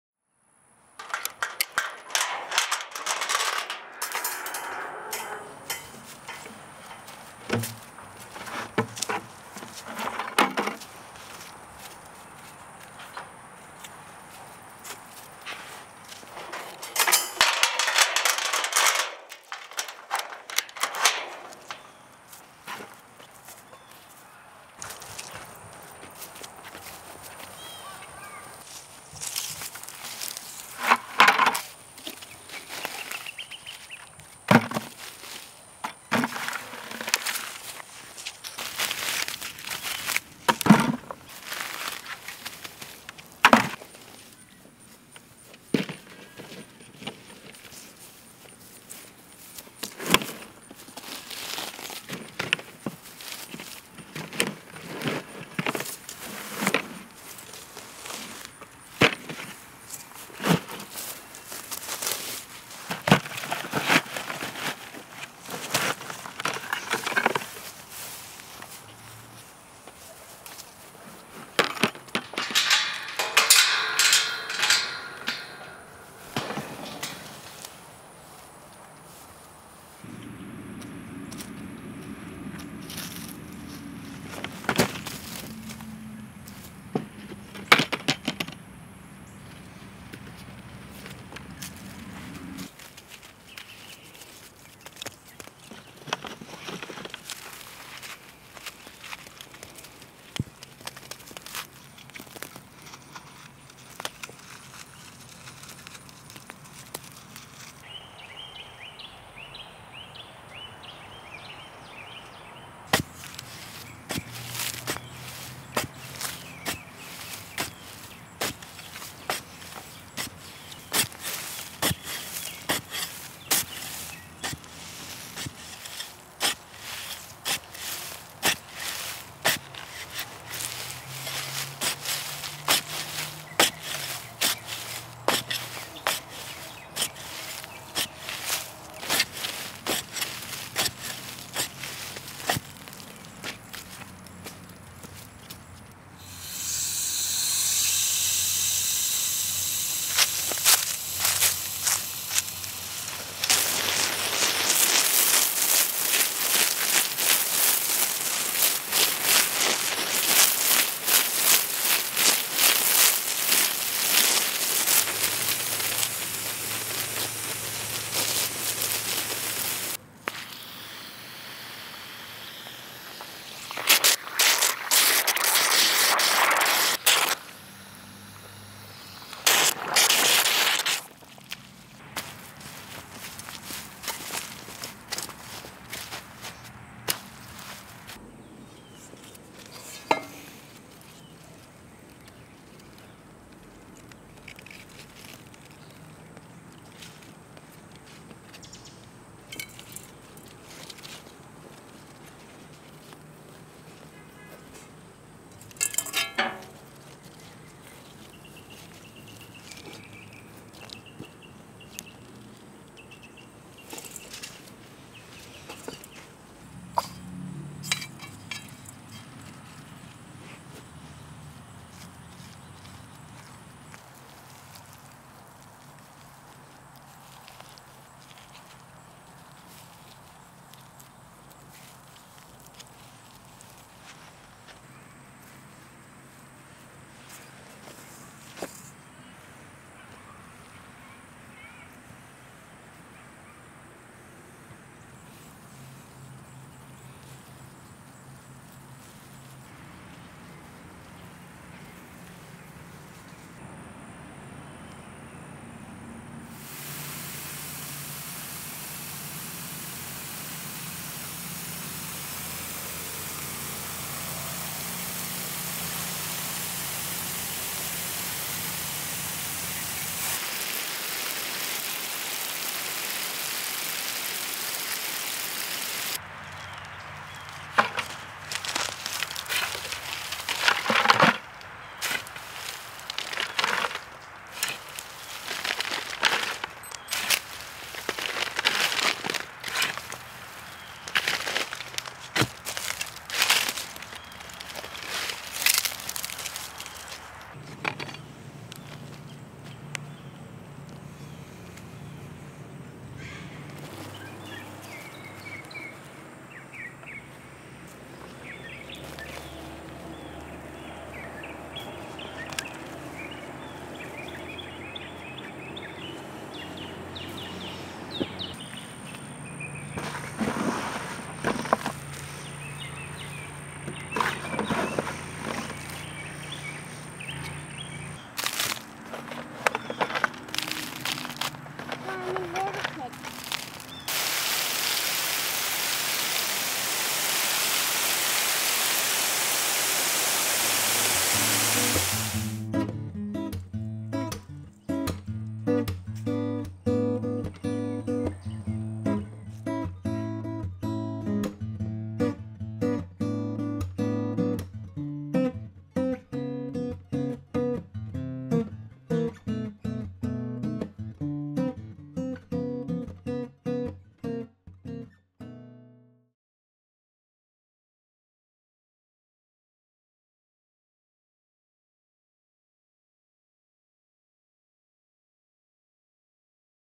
Simple Gardening
Grow or Die

Today we make a simple garden bed.